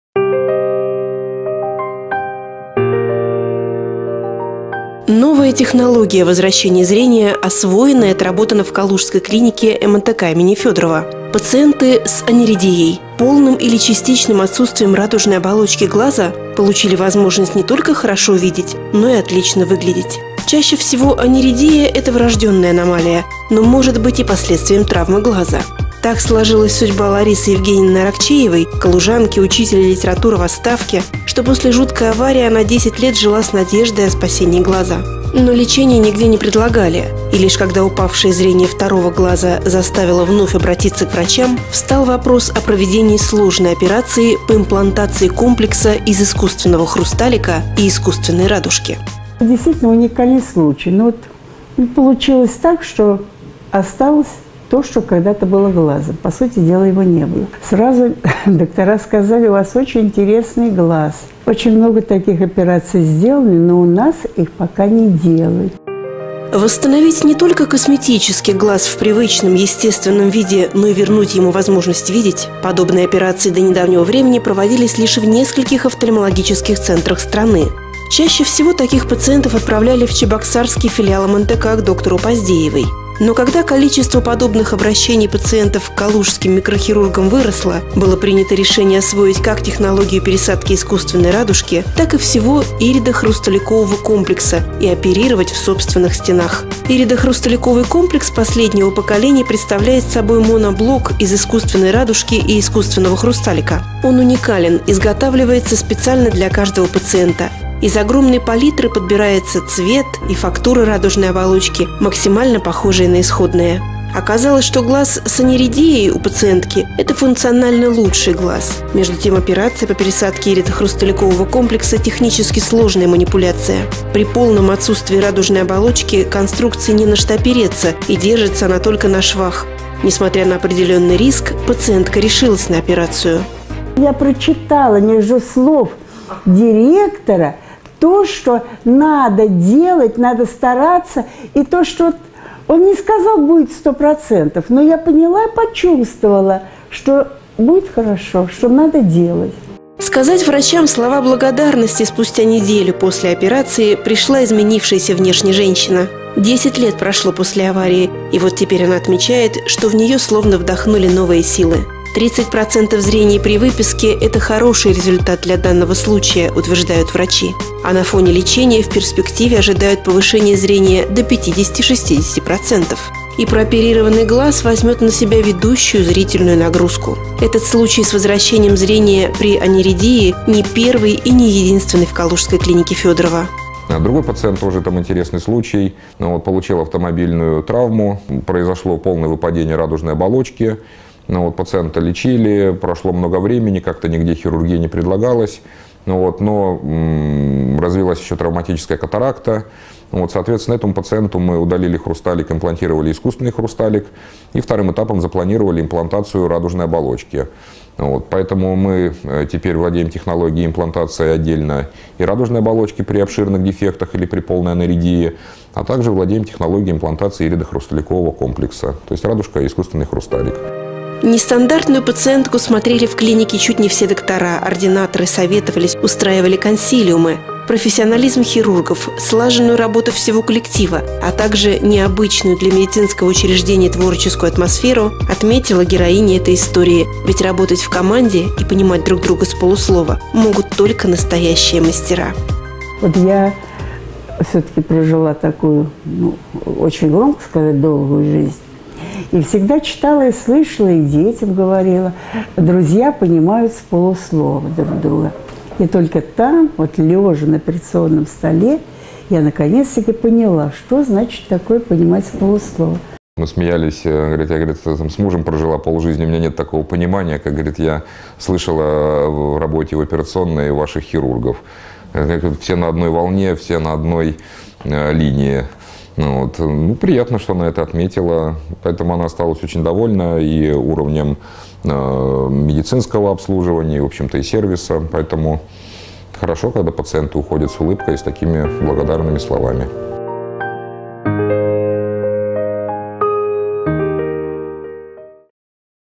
Лечение аниридии в Калужском филиале МНТК. История пациента
Новая технология возвращения зрения освоена и отработана в Калужской клинике МНТК имени Федорова. Пациенты с аниридией, полным или частичным отсутствием радужной оболочки глаза, получили возможность не только хорошо видеть, но и отлично выглядеть. История одного из пациентов Калужского филиала МНТК.